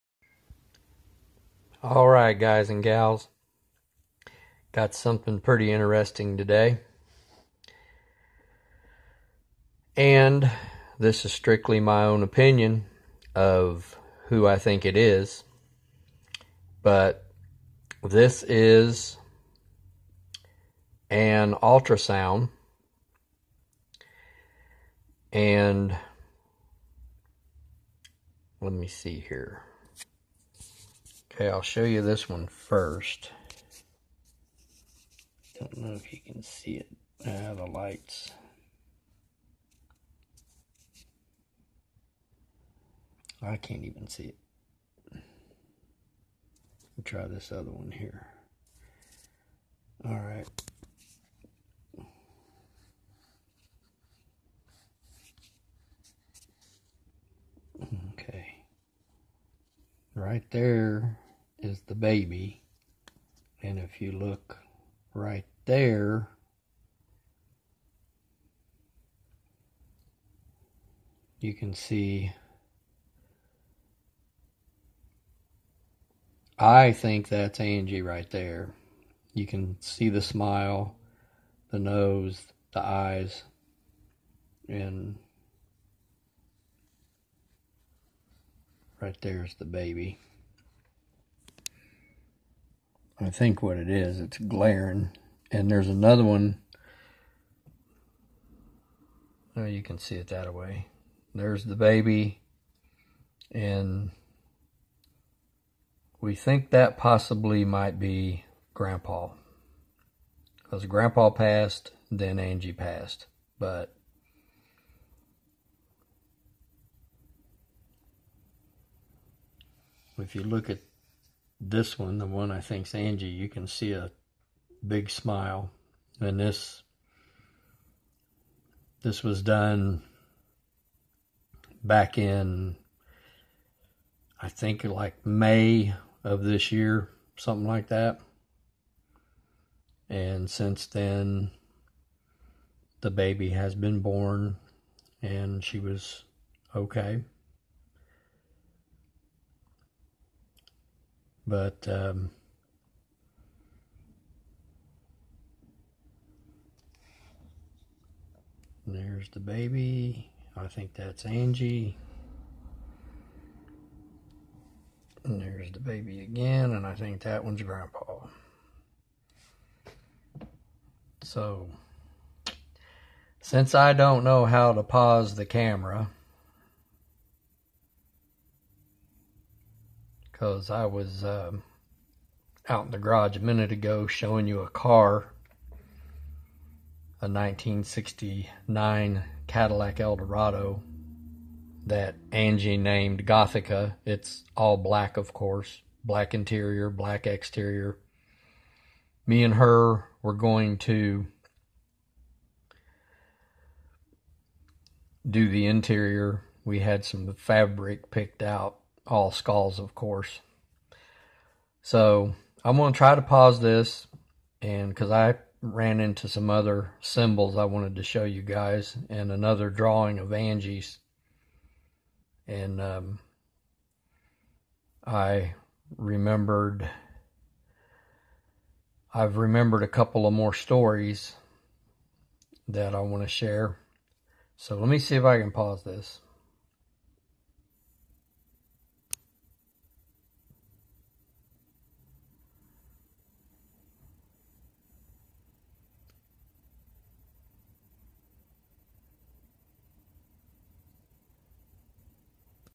not for kids must be 18Nana Angie in the ultrasound
This is our daughter's ultrasound and I believe that was Grandpa and Angie that I pointed out to you in the ultrasound